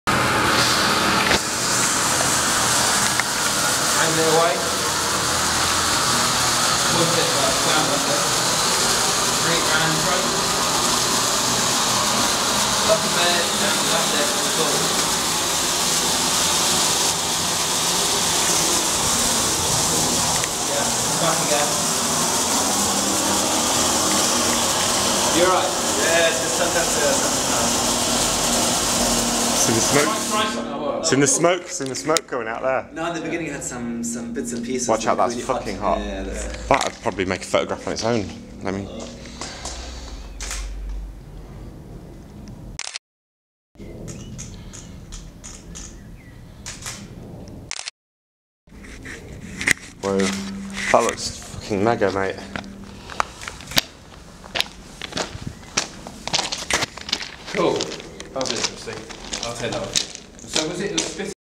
Stills shoot with marine flares
Video of a stills shoot using marine distress flares in a disused railway tunnel in Highgate, London.

Filmed by Stephen Matthews.